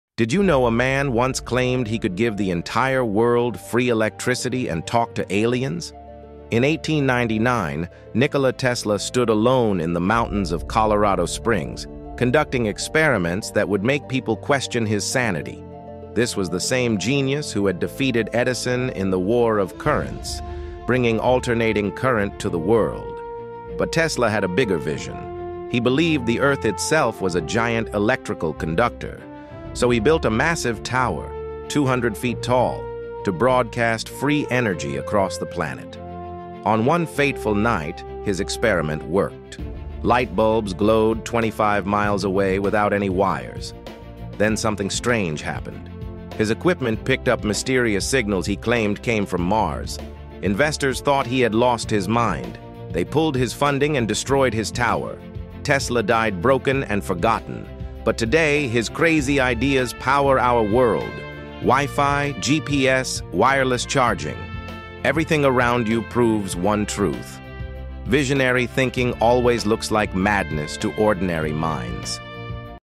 Tesla's Secret: The Man Who Almost Gave the World Free Electricity
Tesla was basically magic ⚡